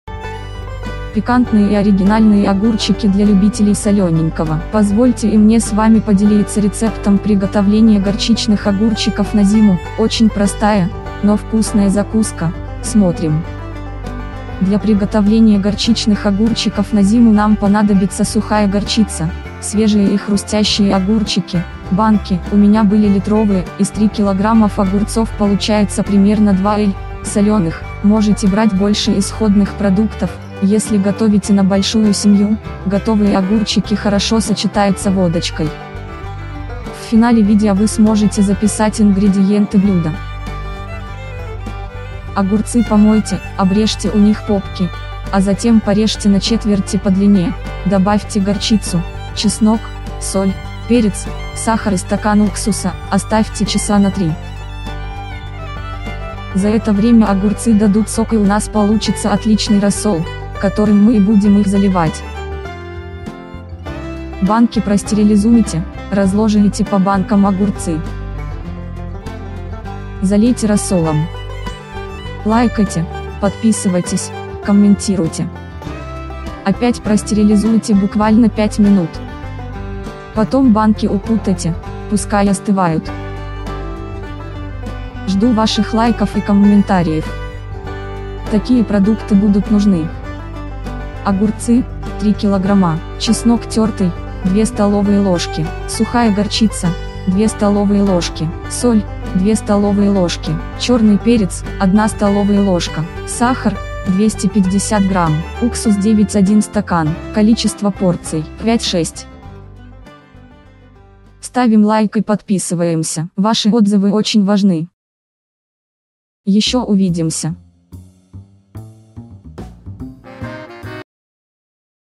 Горчичные огурчики на зиму Рецепт, пошаговый, как приготовить, видеорецепт
Не рецепт, а золото. Пикантные и оригинальные огурчики для любителей солененького! Позвольте и мне с вами поделиться рецептом приготовления горчичных огурчиков на зиму - очень простая, но вкусная закуска. Смотрим!